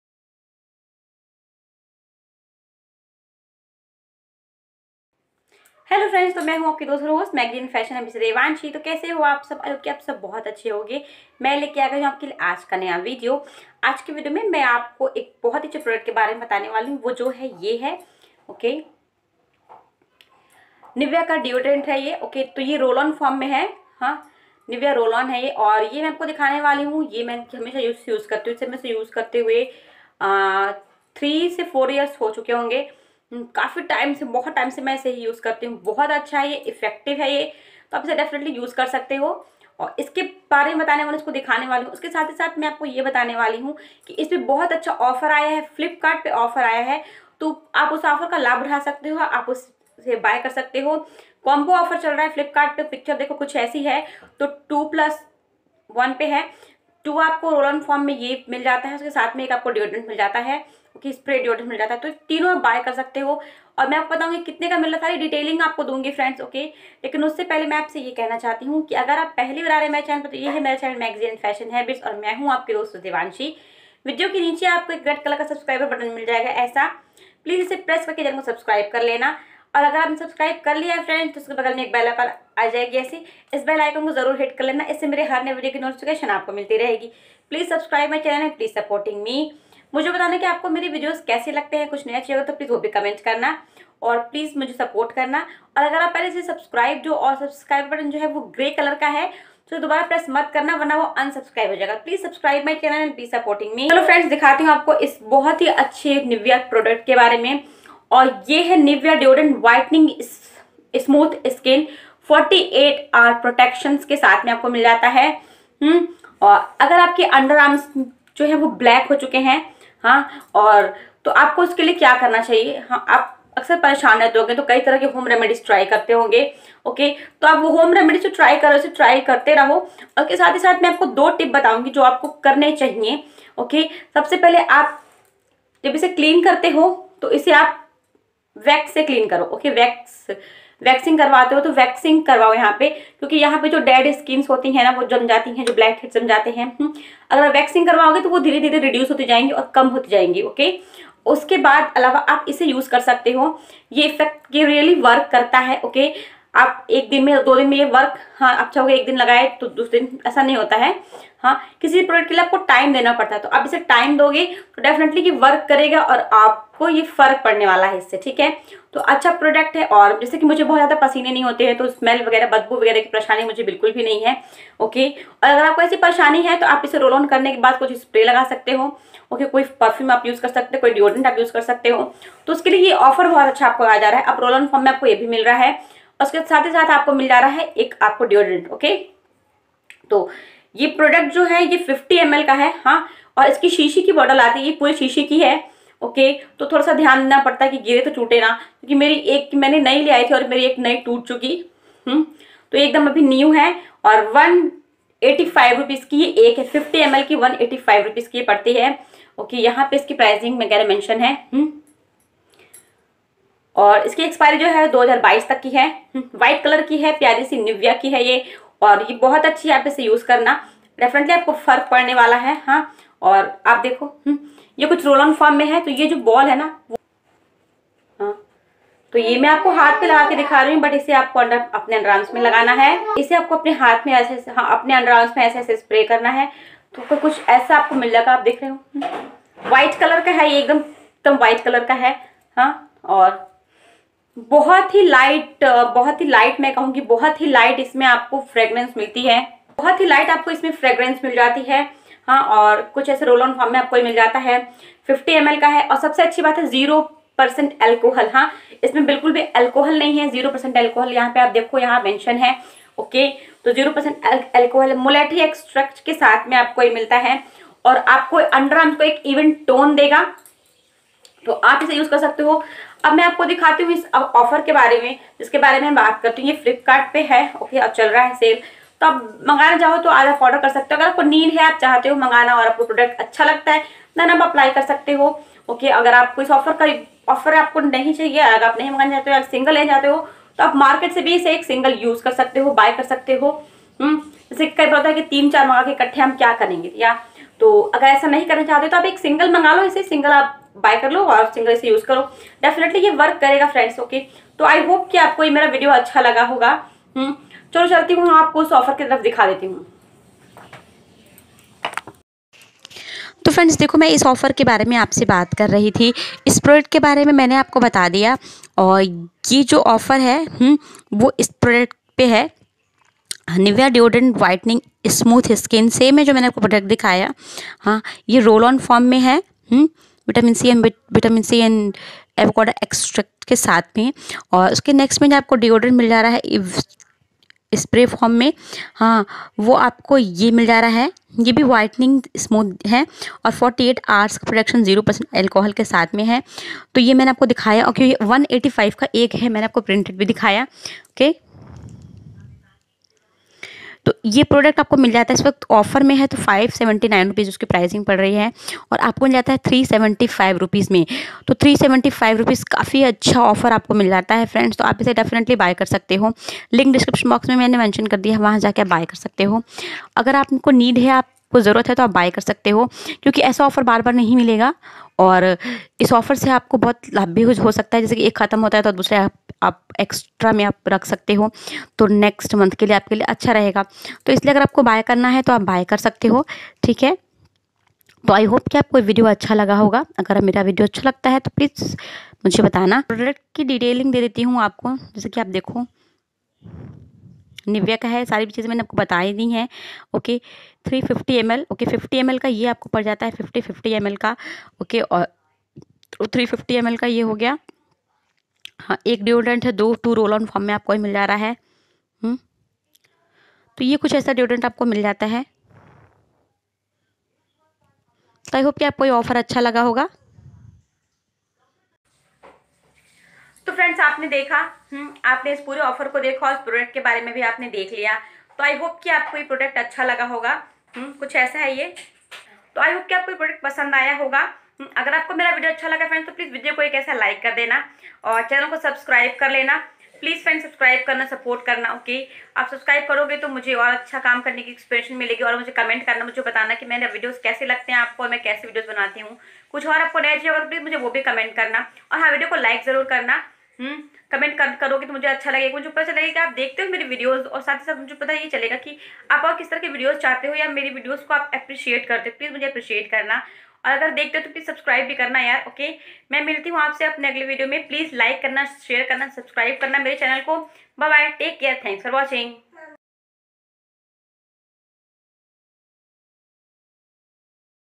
Nivea deodorant whitening roll on review/COMBO OFFER Flipkart huge discount offer
Hello friends.. Aj ke video mein aapko Nivea deodorant whitening roll on review share kar rahi hun. Flipkart pe combo offer acha h vo v share karugi. Video pasand aata hai to LIKE kare. Thanks.